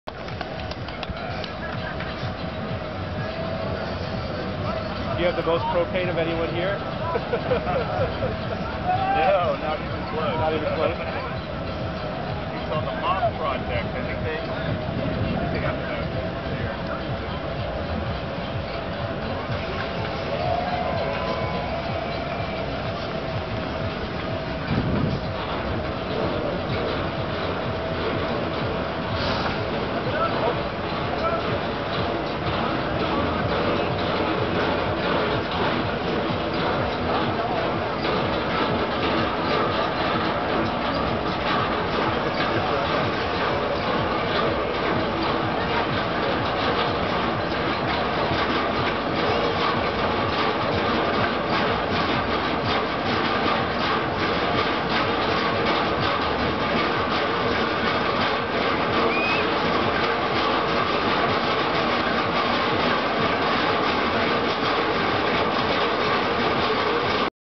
Maker Faire Bay Area 2017: Vortical Fire Ball Zoetrope
Ryon Gesink's propane-fueled, spinning spherical creation is a zoetrope called "Vortical."

Ryon is a custom metal fabricator and large scale artist based in Oakland, Calif.